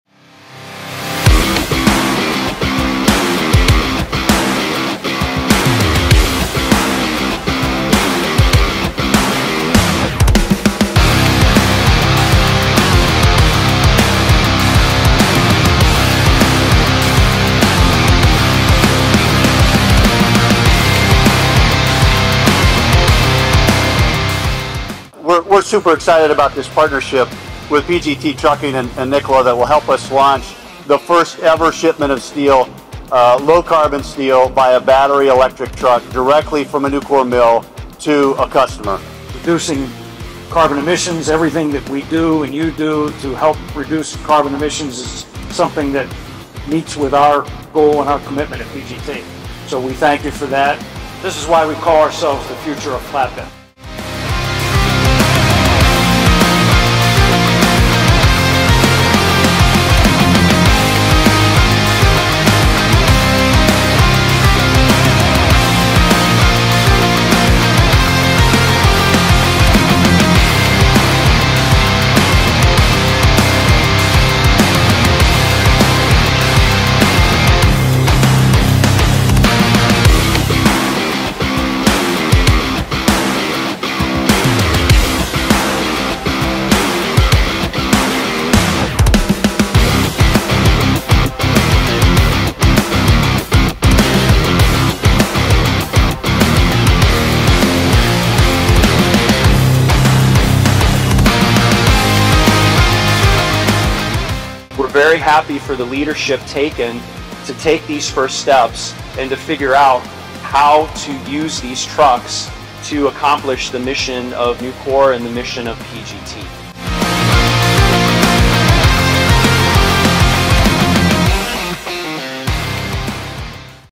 SUSTAINABLE SUPPLY CHAIN IN ACTION
In May 2023, PGT Trucking delivered our first shipment of low-greenhouse-gas steel via Nikola’s Class 8 battery-electric vehicle from Nucor Brandenburg to a nearby fabricator. The shipment was an example of PGT’s sustainable shipping solutions program, offering zero-emission transportation services by using the Nikola Tre BEV truck. Nucor, an industry leader in sustainable steel and steel products, hosted a commemorative event to celebrate the shipment at their new plant in Brandenburg, Kentucky, which specializes in the production of low GHG intensity steel. Company team members, media and local officials enjoyed a behind-the-scenes look at the sustainable equipment and commodities that are transforming the steel and transportation industries.

According to a study by the North American Council for Freight Efficiency (NACFE), if 50% of the heavy-duty regional haul tractors were replaced with battery electric trucks, the industry could save an estimated 29.4 million metric tonnes of carbon dioxide equivalent annually.

Through the Future of Flatbed®, PGT is committed to innovative and sustainable initiatives that provide efficient, safe and environmentally friendly transportation solutions to our customers, helping us achieve our carbon emissions reduction goal. Deliveries made using the battery-electric truck are estimated to reduce carbon emissions by 40% and create a 20% savings in energy costs per trip.